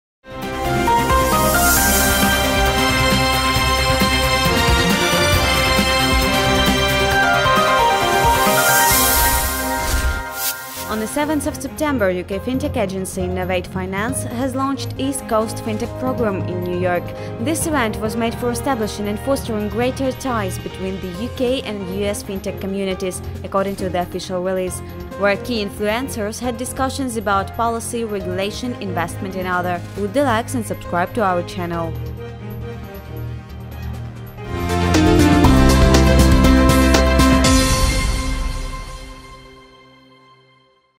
KCN Digest: East Coast FinTech programme was held in NY
On the 7th of September UK fintech agency Innovate Finance has launched East Coast FinTech program in New York. This event was made for establishing and fostering greater ties between the UK and US fintech communities, according to the official release, where key influencers had discussions about policy, regulation, investment and other.